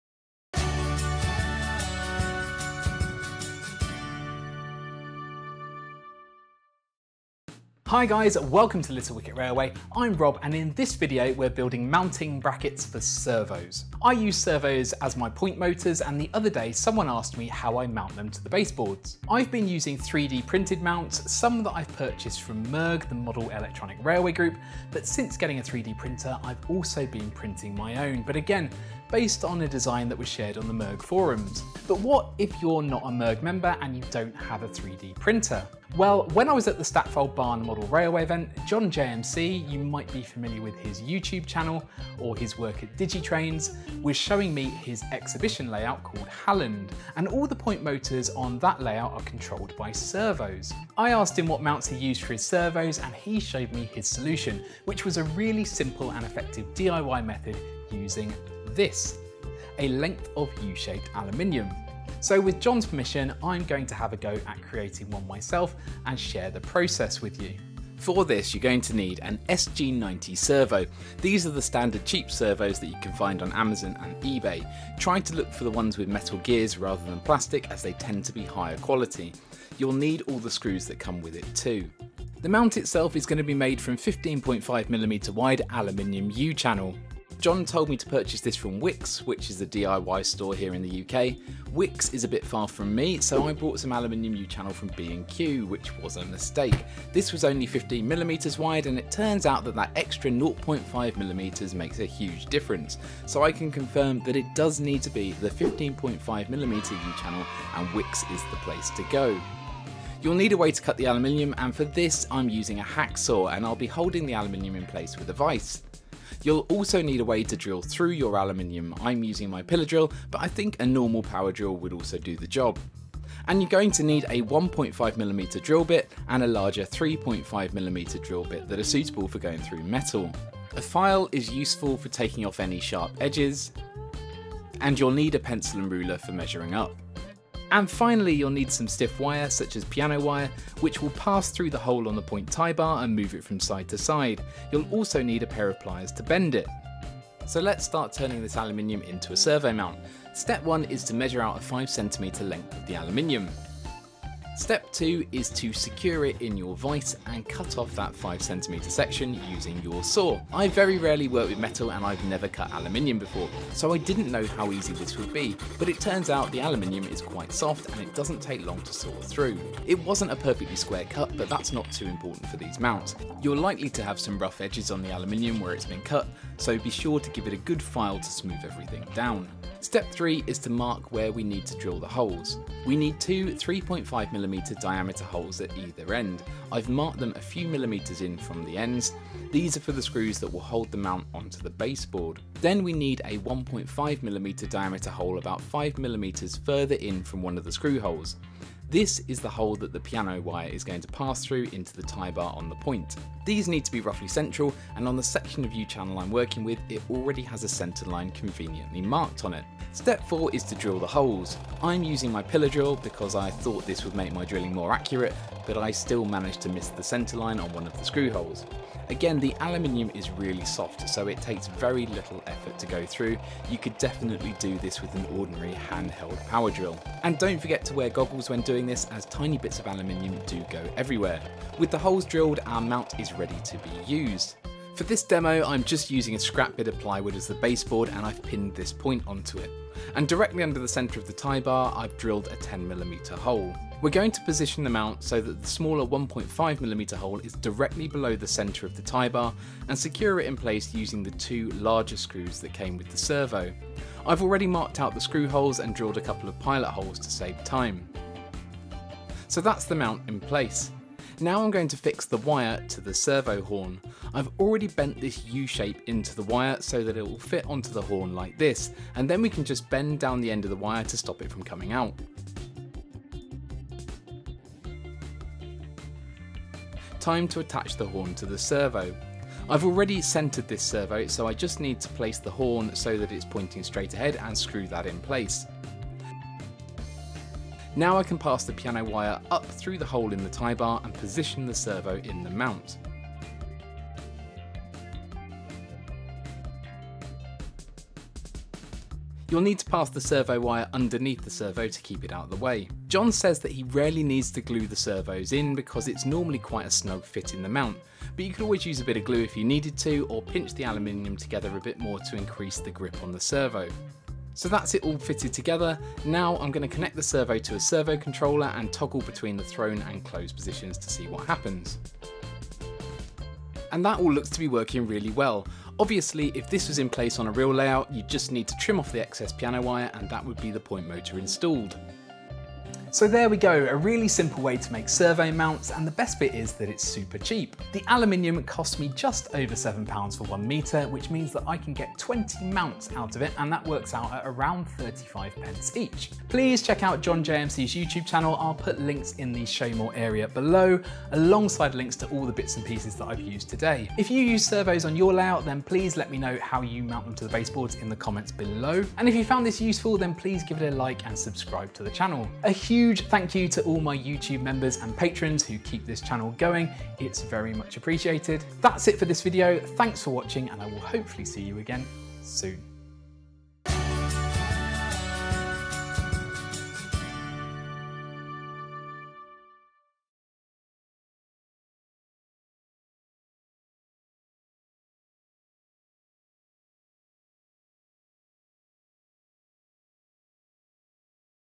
DIY Servo Mounts - Cheap and Easy Solution
A cheap and easy way to make servo mounts from aluminium U section.

00:00 Introduction
01:07 What you need
02:26 How to make it
05:32 Summary